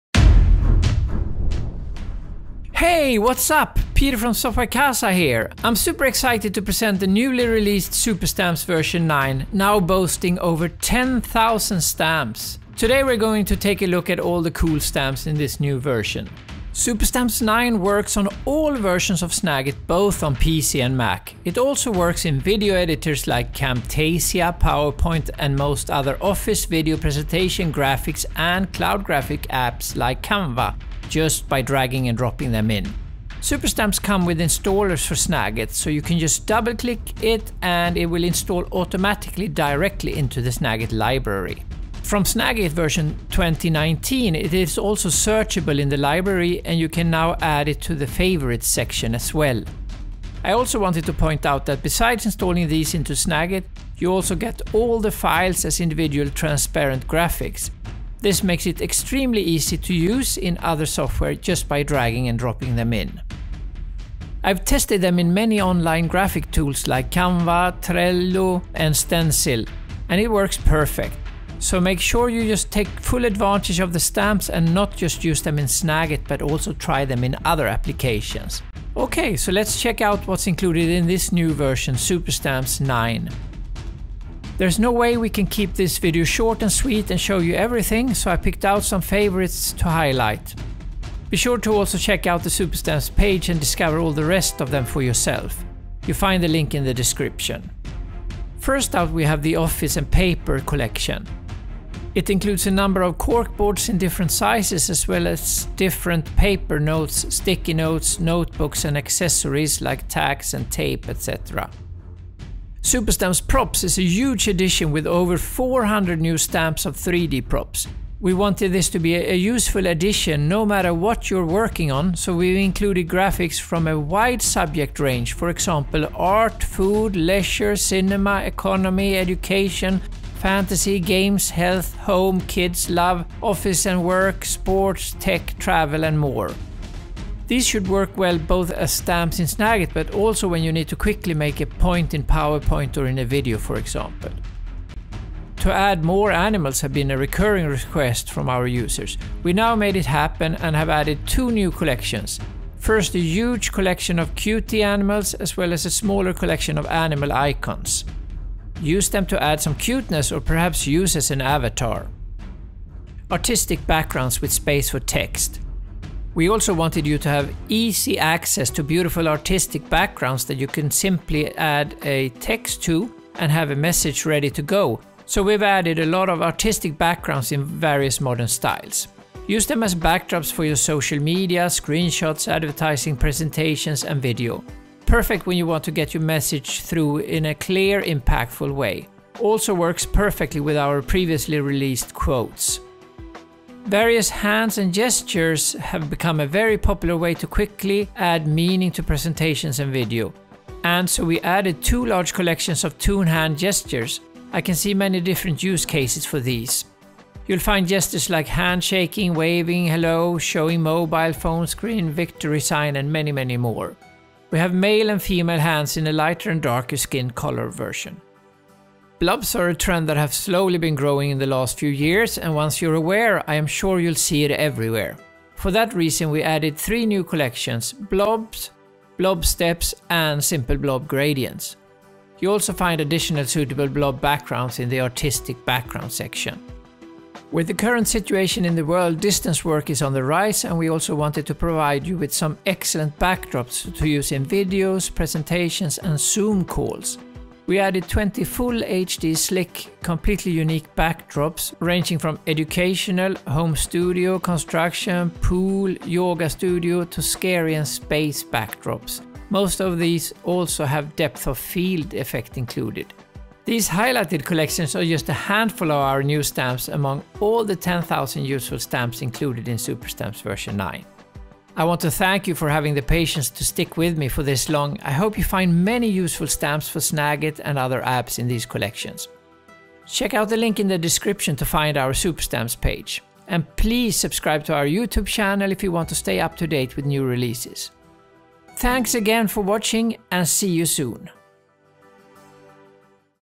10,000 Snagit Stamps - SuperStamps v9 What's Included, new stamps for Snagit
I am proud to present our new Snagit Stamps and graphics collection, SuperStamps v9, check out the full scope

SuperStamps is a huge collection of visual communication assets, created to aid the presenter, teacher, marketer, trainer, etc., to create stunning and professional-looking visual documentation.

The stamps/icons included in SuperStamps cover a wide variety of areas.

The stamps also offer great versatility, you can install and use them directly in Snagit (installer included), but you can also use them directly in virtually any graphics, presenter, or video software, including cloud apps such as Canva, online presentation, and collaboration tools etc., etc...

You'll find SuperStamps v9 jam-packed with useful cool stamps. Developed with and for our clients, including the most useful, professional, and nicest looking stamps.

These are just a few of the new categories. In the video, we're showcasing some highlighted new collections, and please check out our product page to find all included stamps.

Peter Gillberg
SoftwareCasa